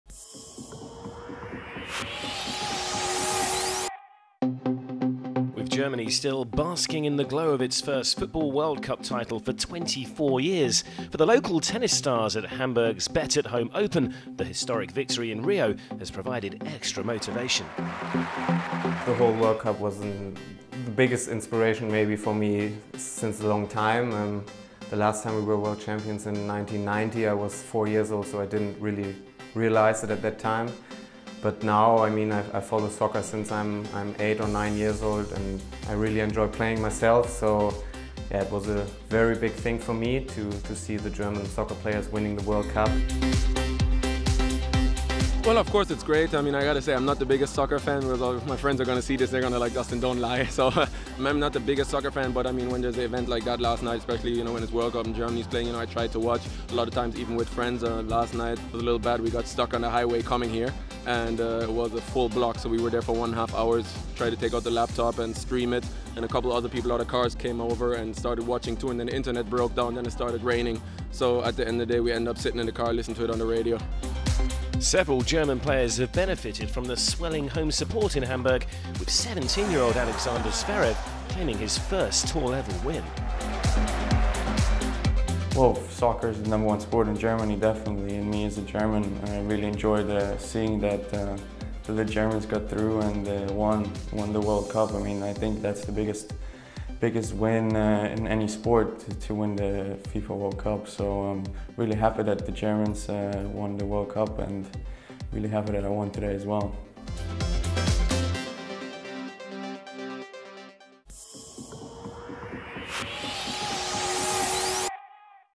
Hamburg 2014 Wednesday Football Feature
Tobias Kamke, Dustin Brown and Alexander Zverev, who are competing in Hamburg, react to Germany winning the football World Cup.